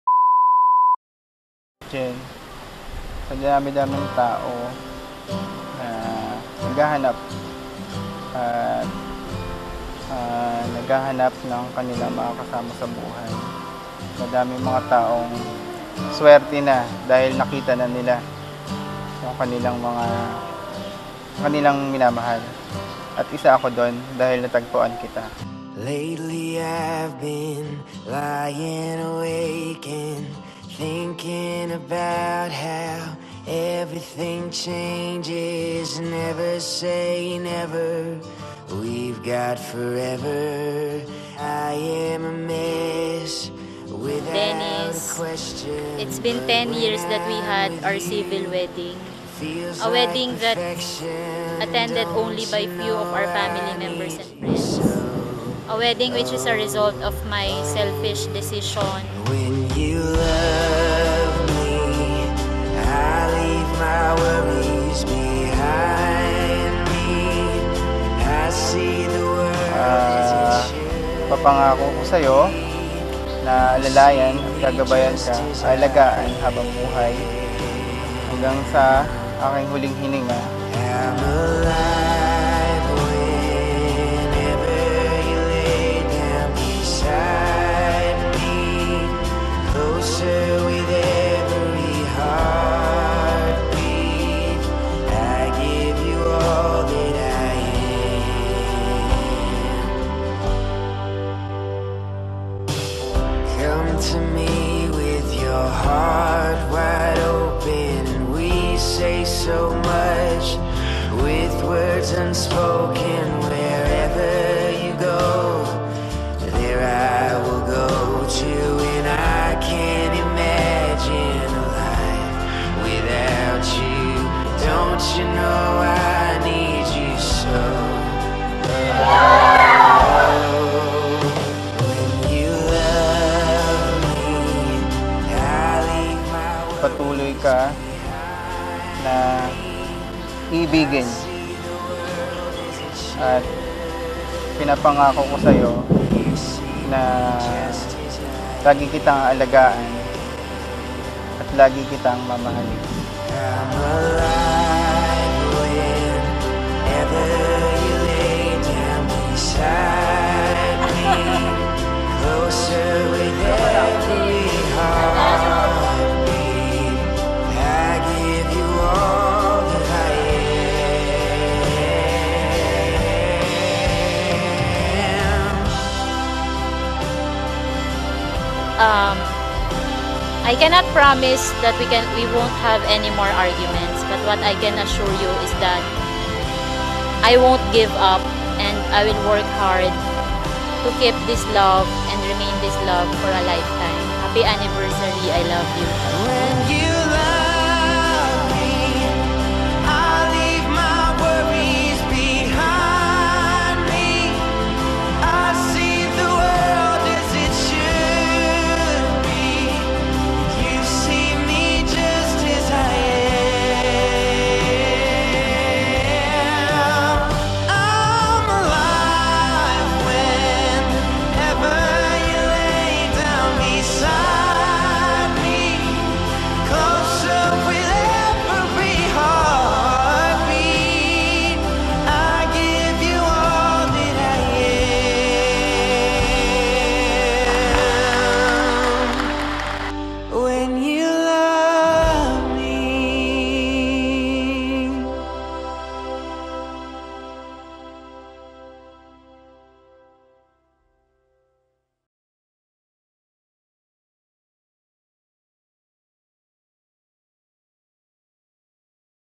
Dennis & Jen "SDE" 06.28.13 - KIKO
Rates, Prices, Overview. Contact usChurch : Sta Clara De Montefalco

Prep : Linden Suites
Reception : Riverfront Residence Clubhouse
Pre Requisite to shoot @ Monteflaco : Seminar & Accreditation

Production Team : Rence Bautista / Sahad / JBR / Franz Francisco